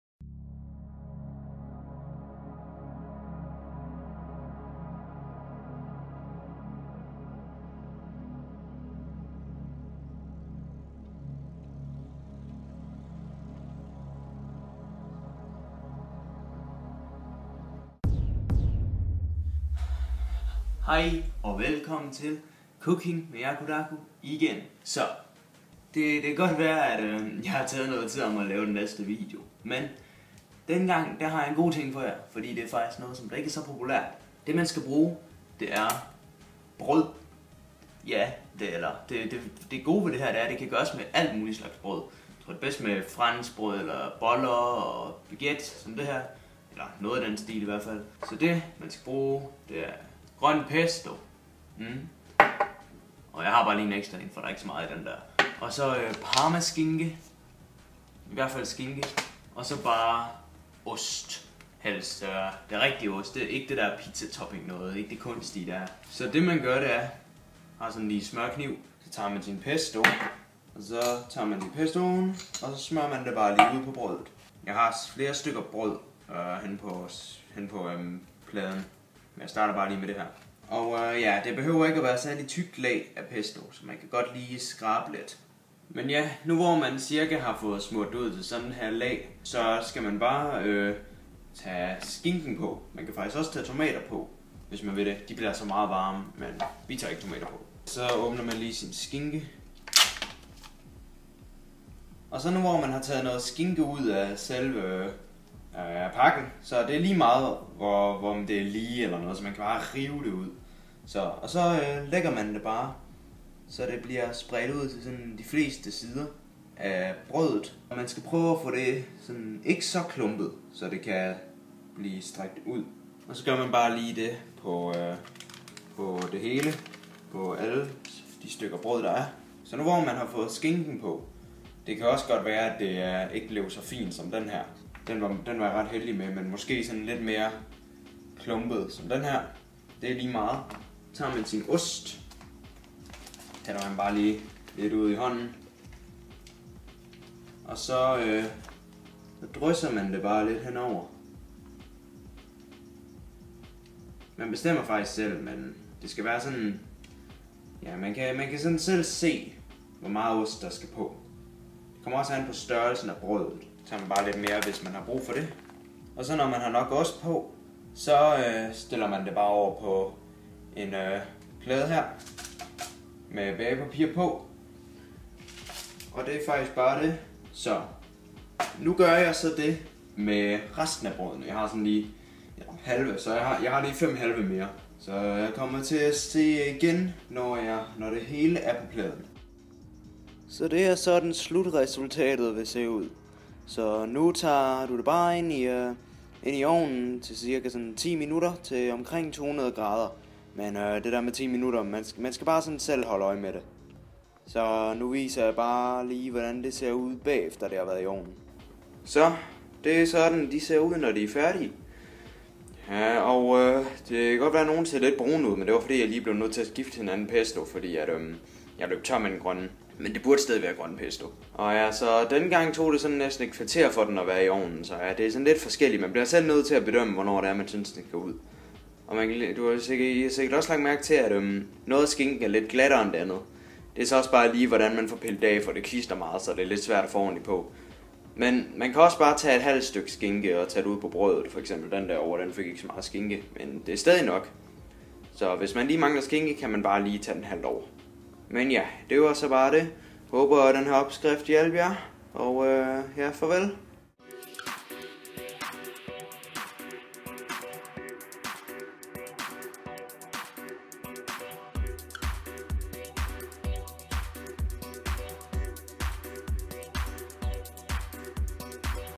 Bruschetta | Cooking Med Jakodako #2
(3-4 års gammel video lol)
I denne udgave af "Cooking Med Jakodako", vil jeg vise jer hvordan man kan lave den lækre snack/ret: "Bruschetta"

Det fede ved denne ret er, at man kan bruge nok en hver slags brød til retten. Man kan også bruge rød pesto, hvis man foretrækker det

Det er også en meget mættende snack, fordi der er ret meget fedt i pakket ind i fyldet, men det smager mega godt :D